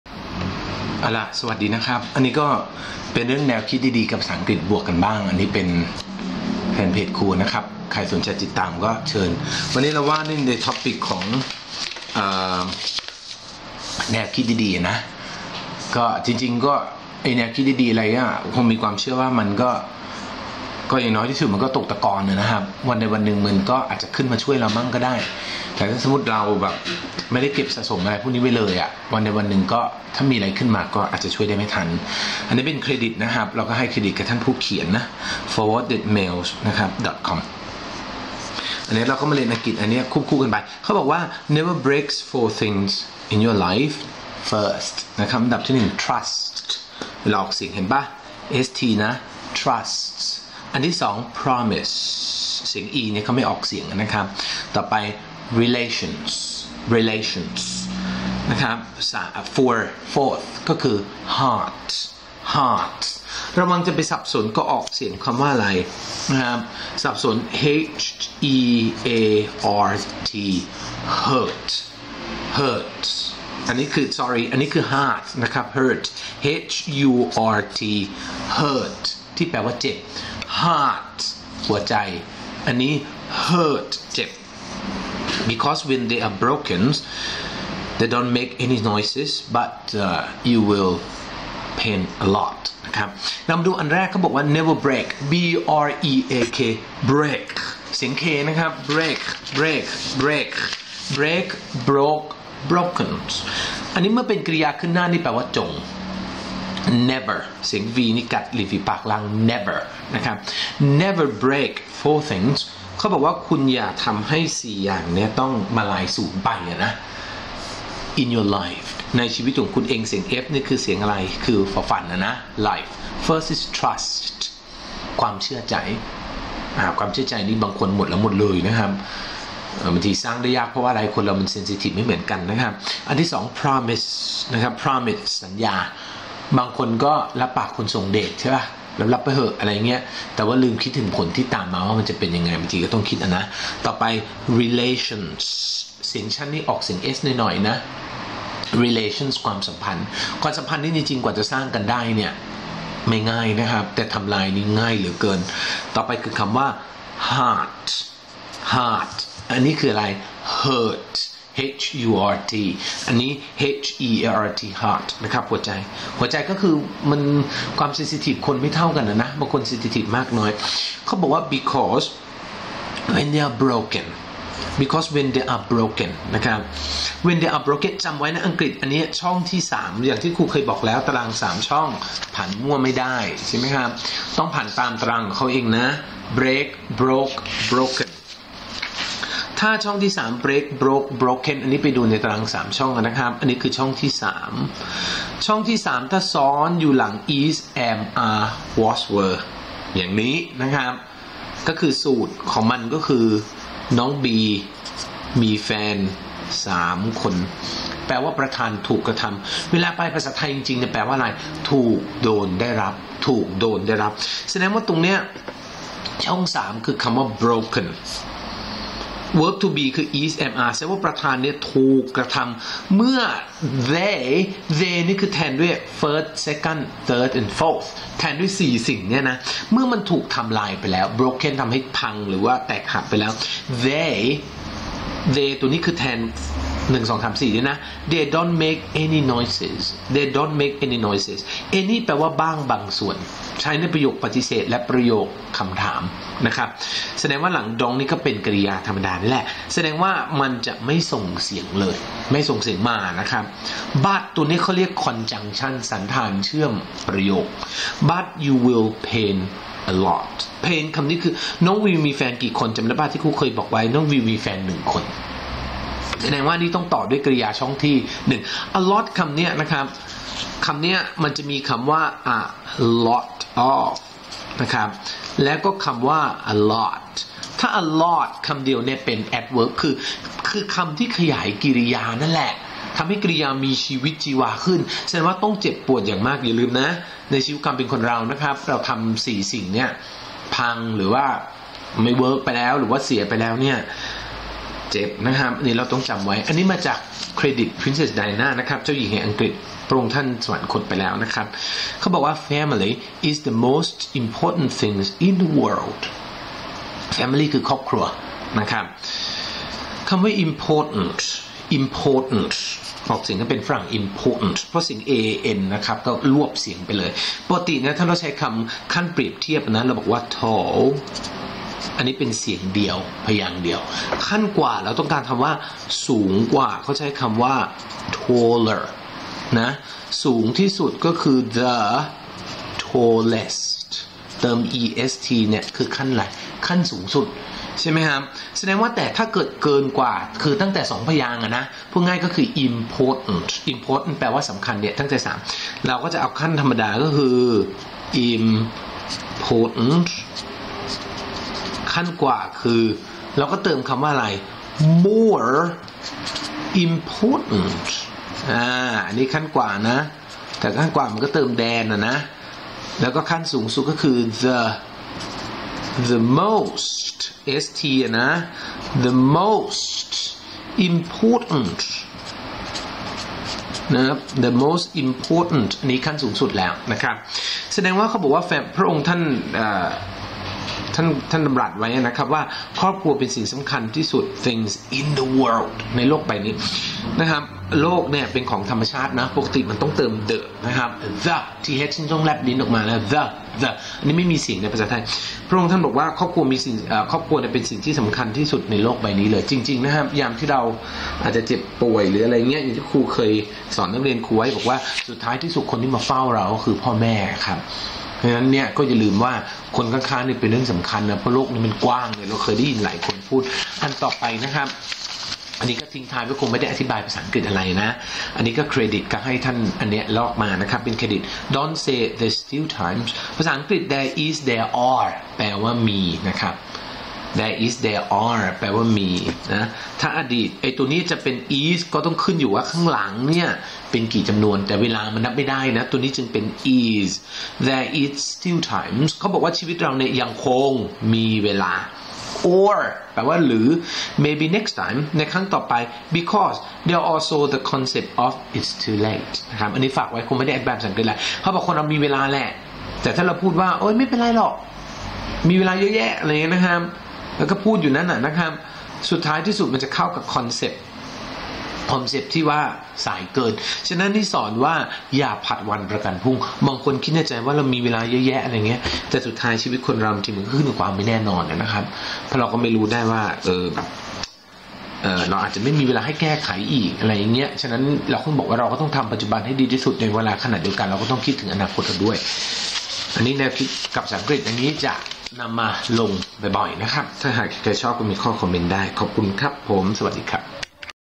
แนวคิดทางภาษาอังกฤษดีๆ (โดยครูวสันต์ อายุบเคน)
แนวคิดและภาษาอังกฤษ - ครูวสันต์ อ่าน แปล อธิบายให้ฟัง - ครูจะได้เตือนตัวเองด้วยครับ ...
กว่าจะเป็นผู้ใหญ่ที่นิ่งได้ เชื่อได้ว่า ต้องผ่านอะไรๆมามากพอสมควร ...
ติดตามครูได้ที่เพจครูนะครับ: ครูวสันต์ อายุบเคนเพื่อแนวคิดดีๆและภาษาอังกฤษ